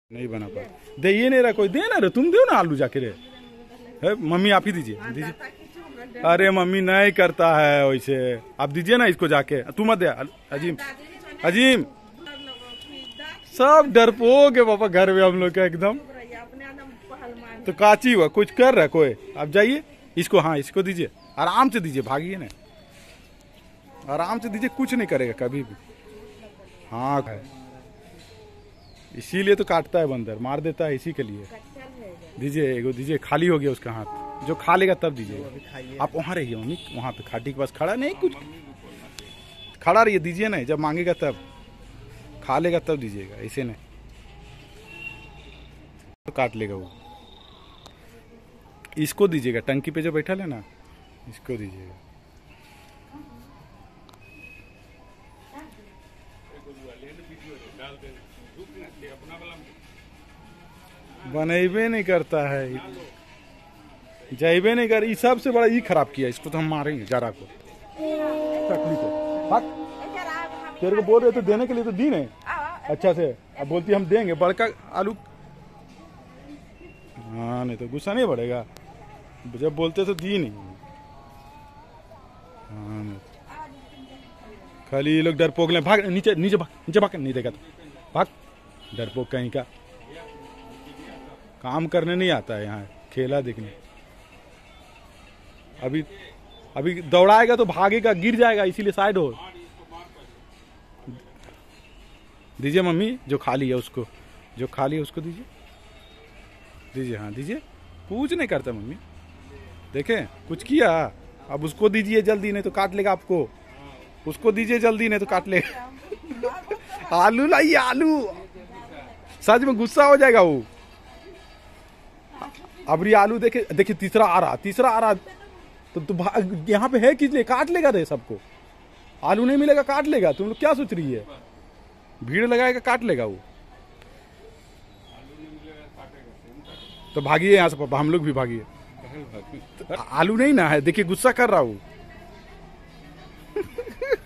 ममी ने भूखे लंगूर बन्दर को आलू खिलाया।
ममी ने भूखे लंगूर बन्दर को आलू खिलाया। ‎@Qadri vlogger